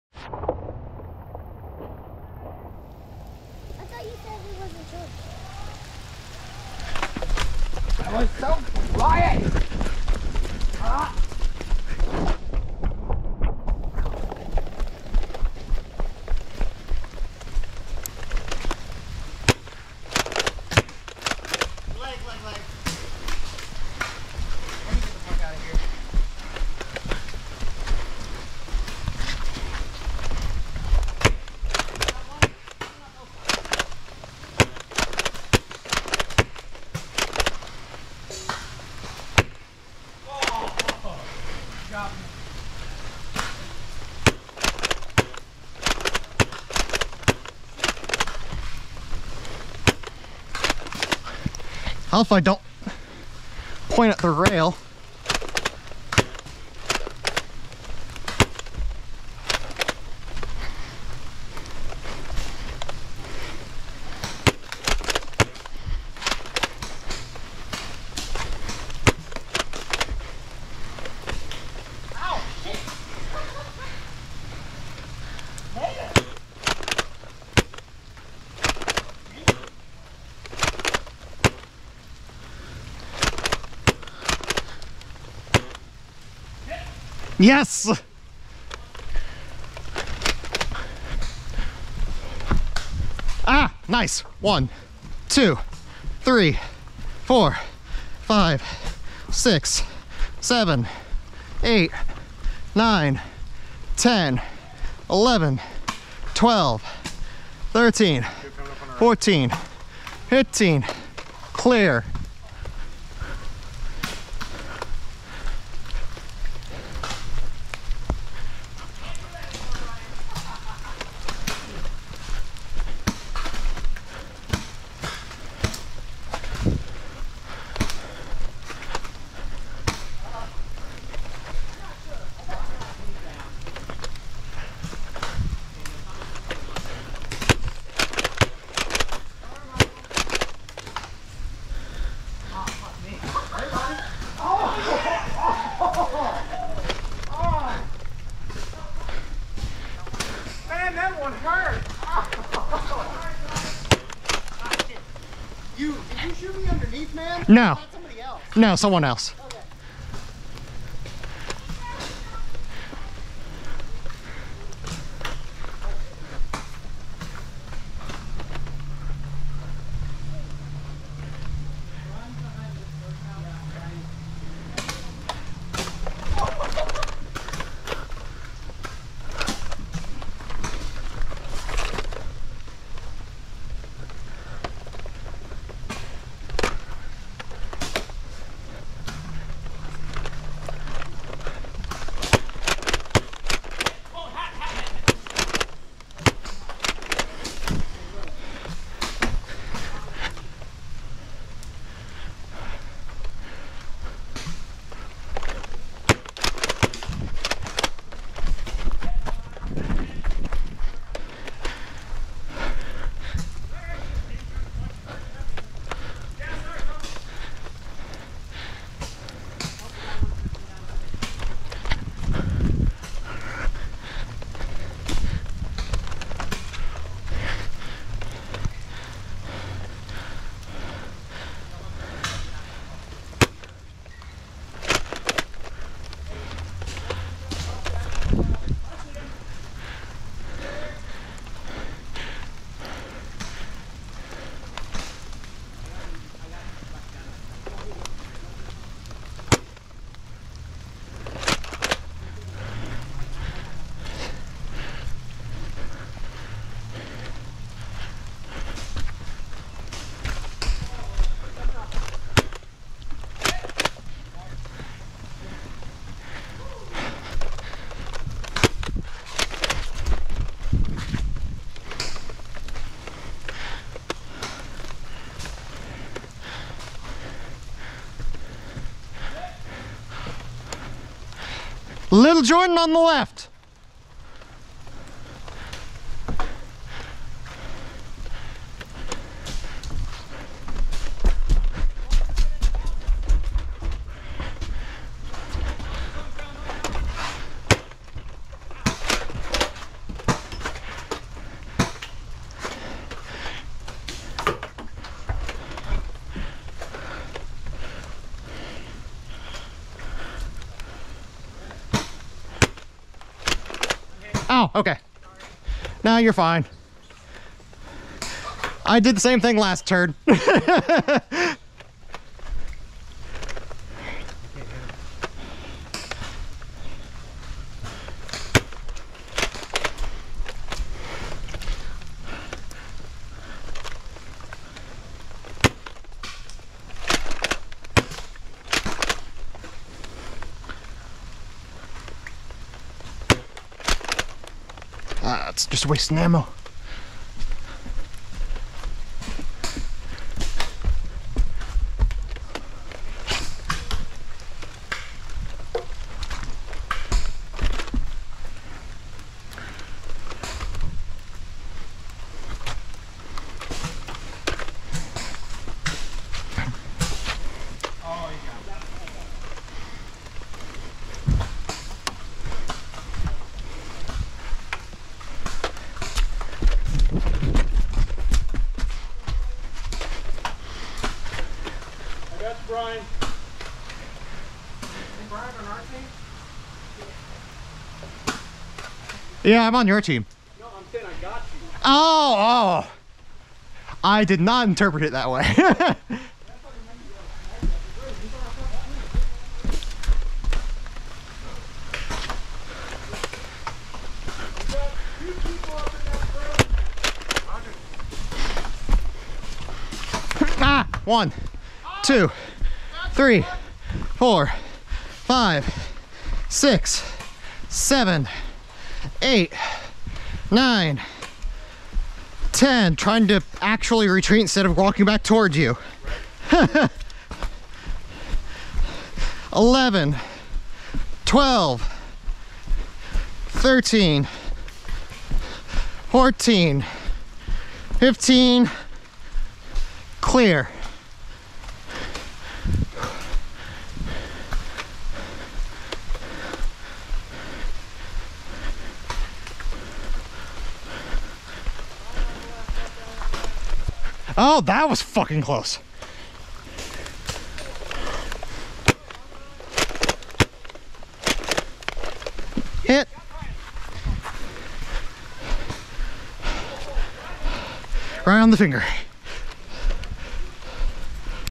Dayton Nerf War - Team Deathmatch with the Dart Zone Max Tomcat
Another round in the rain, but this time with the new cylinder-fed pro blaster from Target. I have yet to write the review, but I've been very happy with it so far.

Also, the dart bouncing off of the ground and then hitting the camera was entertaining.